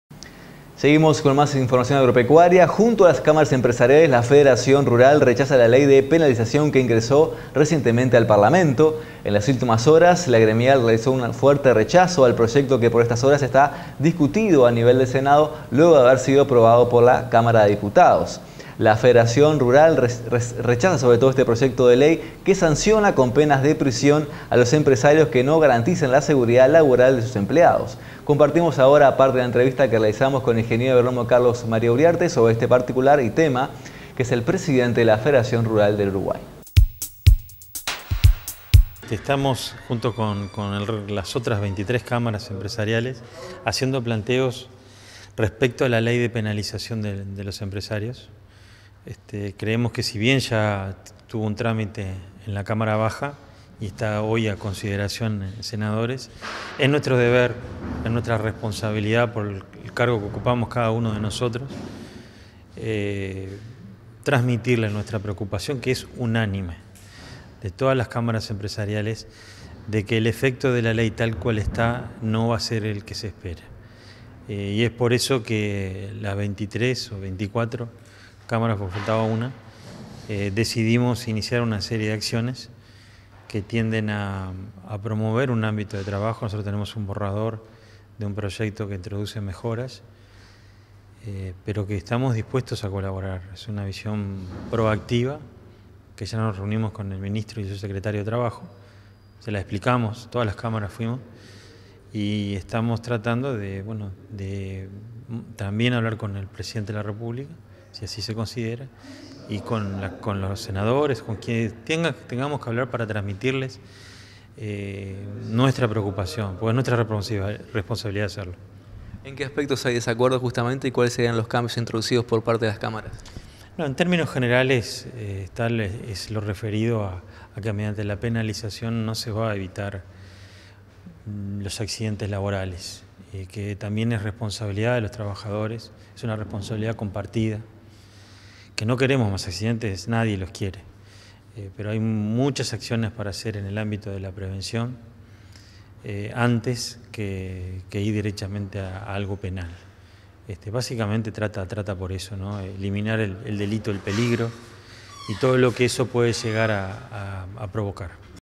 CARLOS MARIA URIARTE
LA FEDERACION RURAL RECHAZA EL PROYECTO DE LEY QUE SANCIONA CON PENAS DE PRISION, A LOS EMPRESARIOS QUE NO GARANTICEN LA SEGURIDAD LABORAL DE SUS EMPLEADOS.